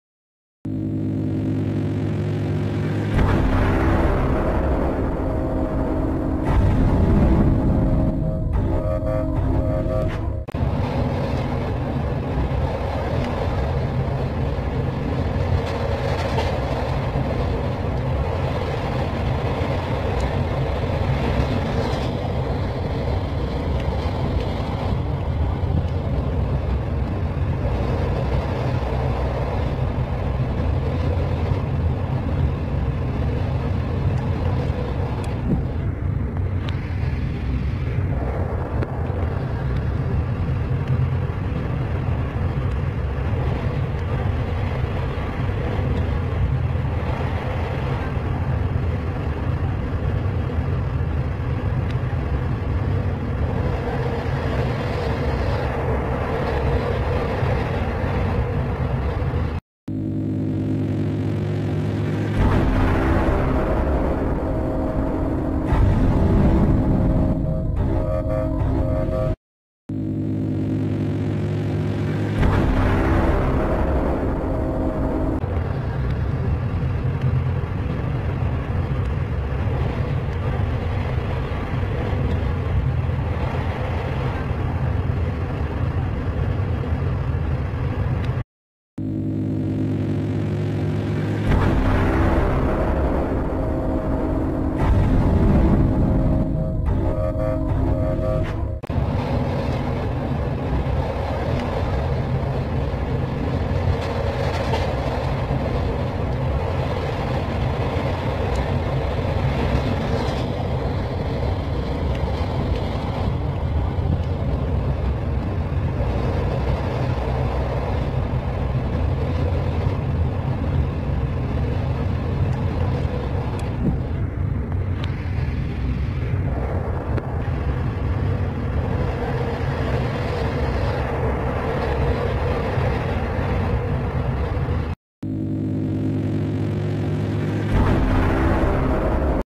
Nibiru: Two Suns Visible Over Japan Planet X Update 2016
Two Suns Footage over Japan April 26, 2016. Mainstream Media News Has recently been asking us if Nibiru Planet X Will cause Mass extinction this month to .

PLANET X UPDATES :Nibiru TWO SUNS footages ~ Planet X - DECEMBER 2016!!PLEASE SHARE!!! Two Suns Footage over Japan April 26, 2016. Mainstream .